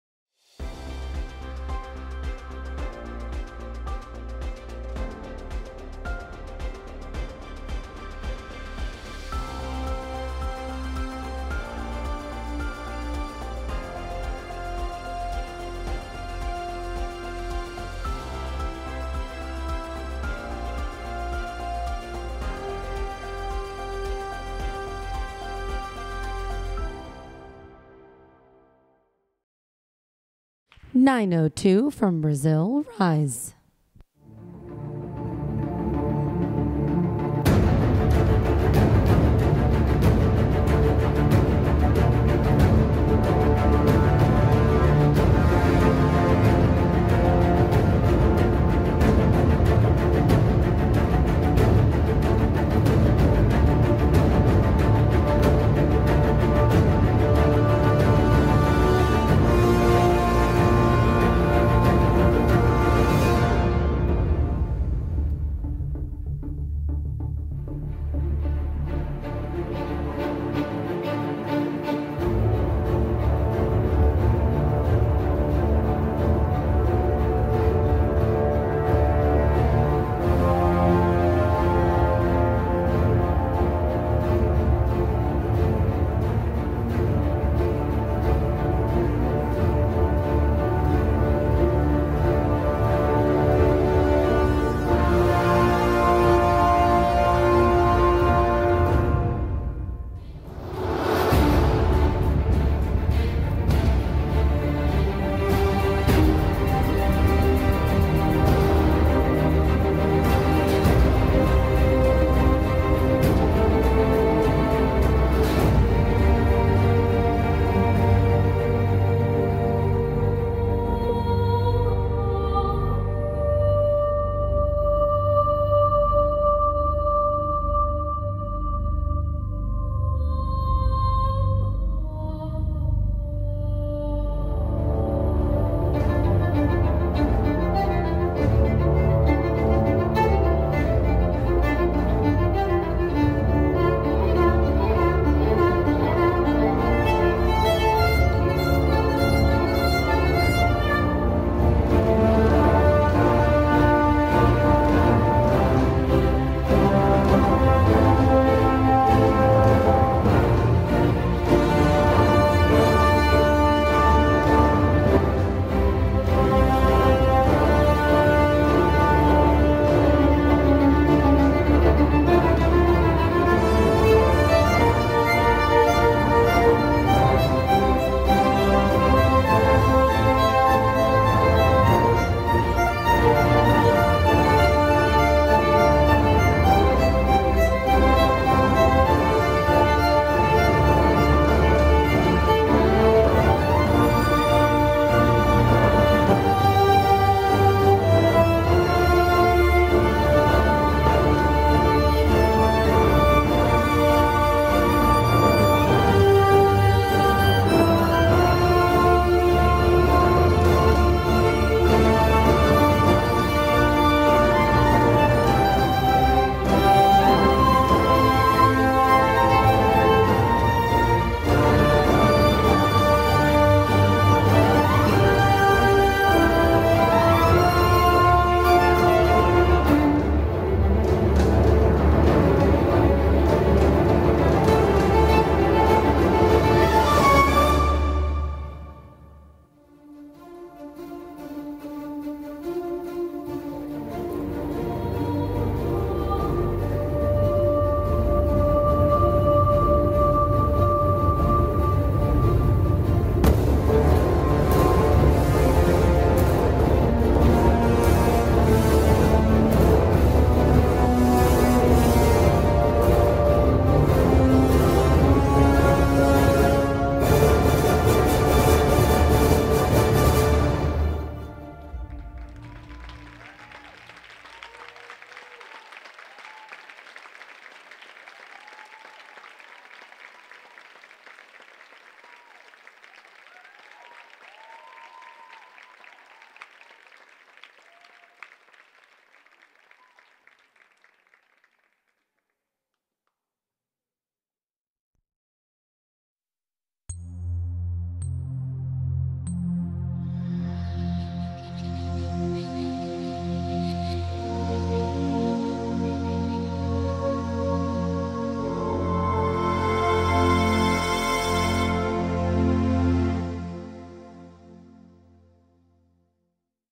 Ballet Vera Bublitz - Rise - YAGP FINALS 2018 NYC
YAGP FINALS 2018 NYC Ensemble Category
Ballet Vera Bublitz
Coreografia: Rise
Coreógrafos: Carlla Bublitz e Marco Sanches